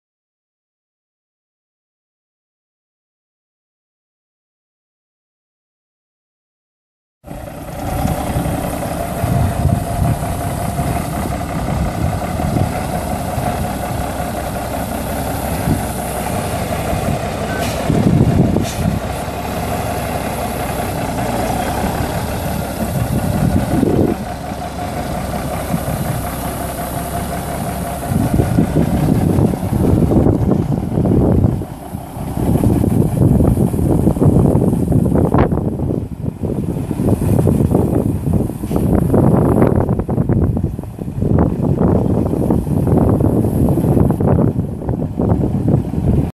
1982 International C09670 semi truck for sale | no-reserve Internet auction October 20, 2016
1982 International C09670 semi truck
892,552 miles on odometer
Cummins 350 six cylinder diesel engine
350 HP
Eaton Fuller Road Ranger 10 speed manual transmission
Tandem axle
Non-operational AC
Air suspension seat
34" sleeper
Single bed
Spring suspension
Air brakes
275/80R24.5 tires
11-24.5 rear tires
No power steering
Last inspected in September 2015
Kansas title
1HTL23273CGA16987

1HTL23273CGA16987

Item location address: This item is located near Spring Hill, KS

It's located in Spring Hill, KS, and we're selling it to the highest bidder regardless of price.